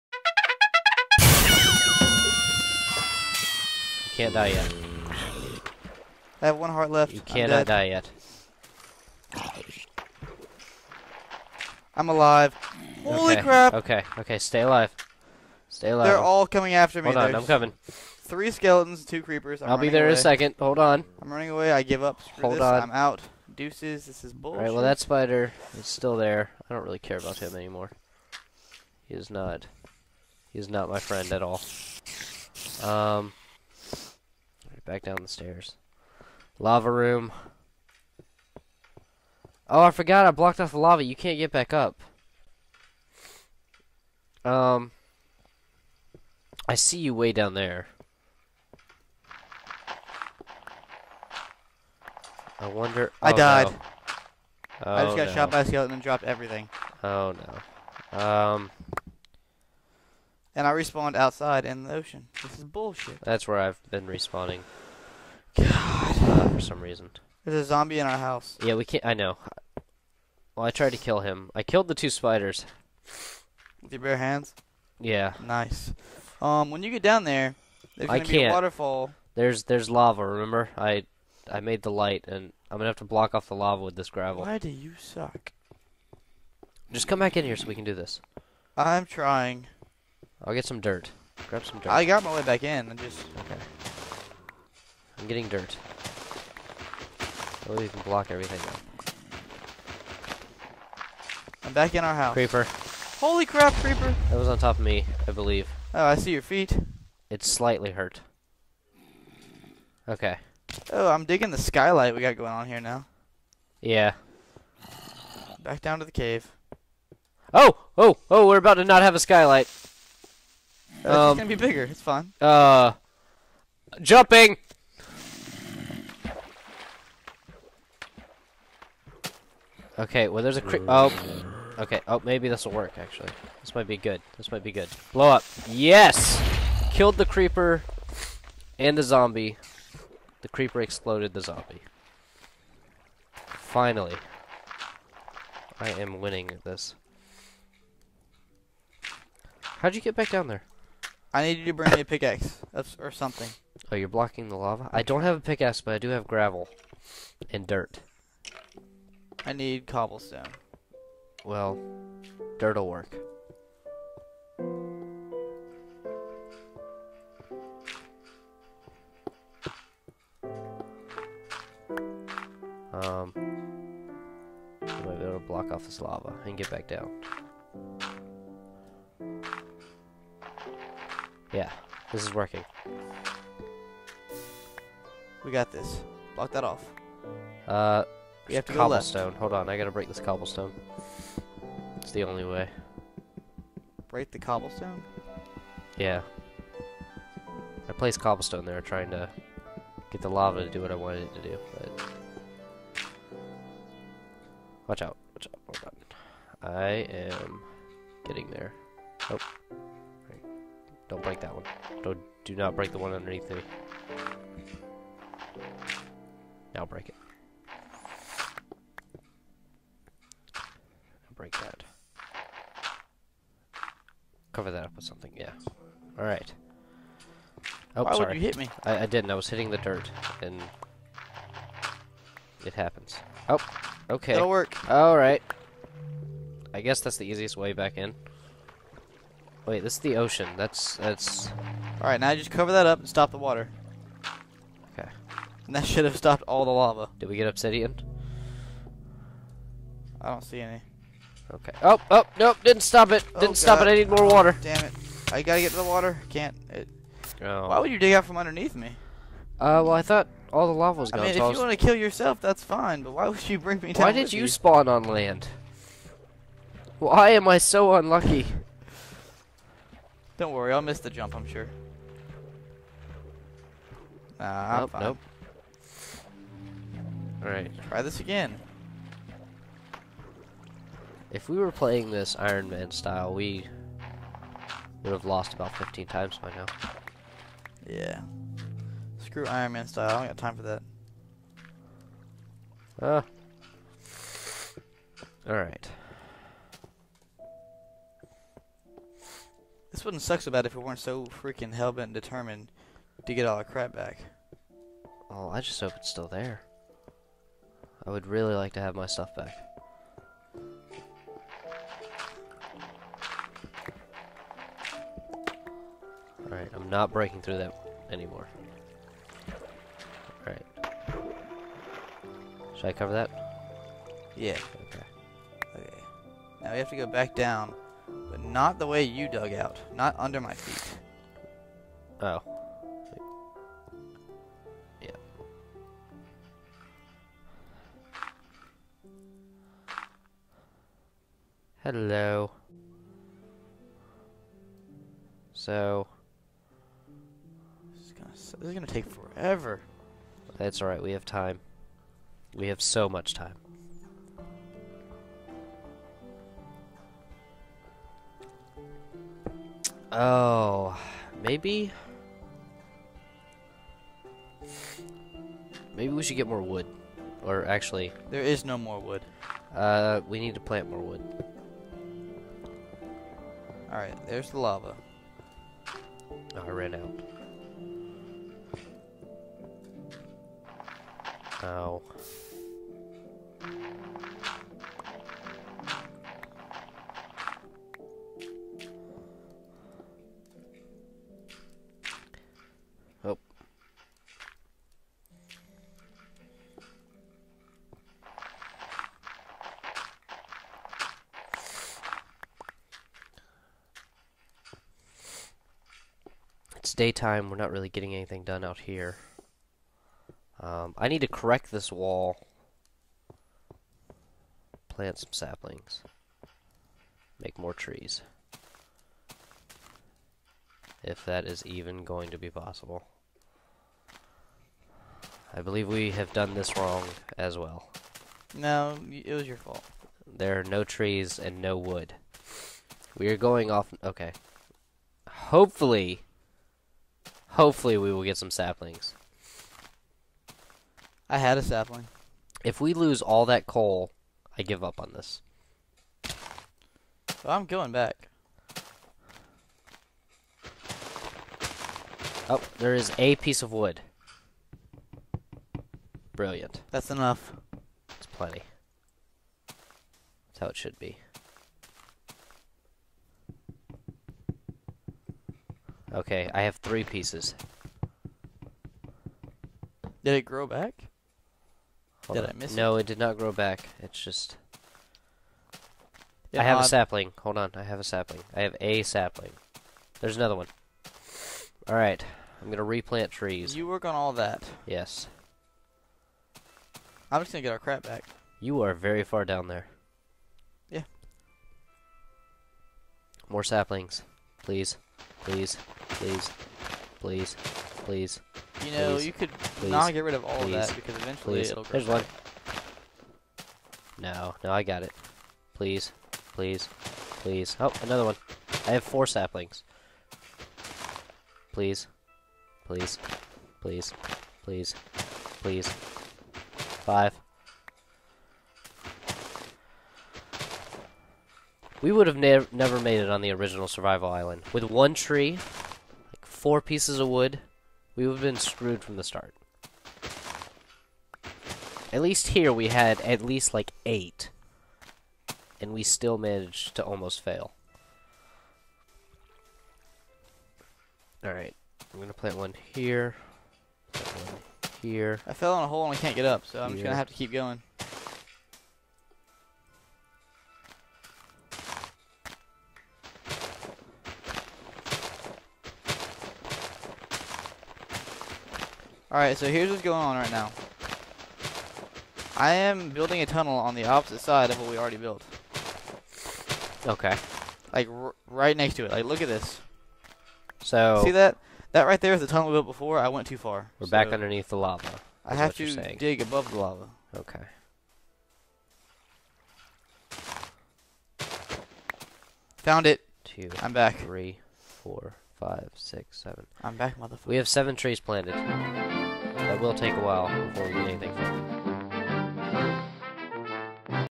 Survival Island Part 6 "Living The Dream"
Yoshi and Eric Casts:

A Survival Island Map that's not as hard as actual Survival Island, but we make it seem that way.

Any suggestions on maps or game types you guys would like to see?

Apologize for the audio, the masks were just there, so we HAD to use them.

Love,
HomicidalYoshiCasts